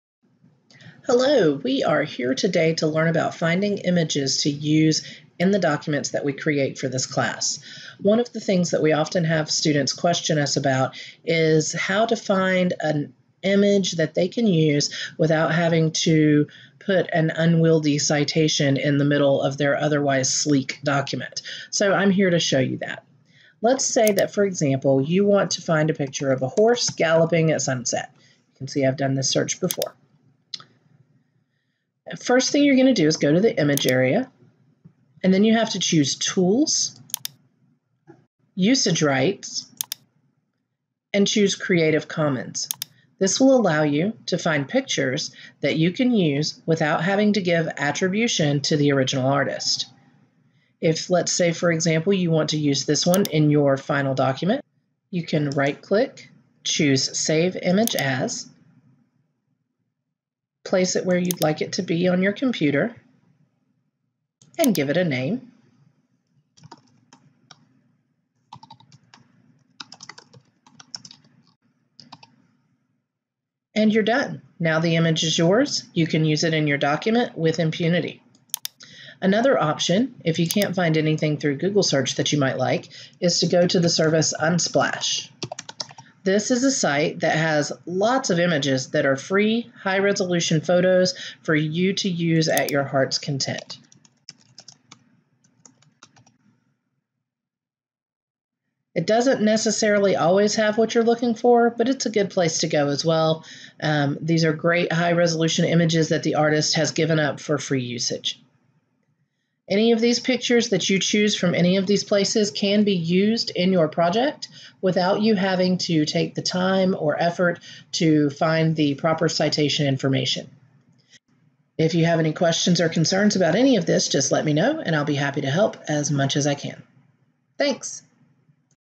Choosing Images | Sasser WRIT 3315
This video discusses how to find images that are free to use without attribution in your projects.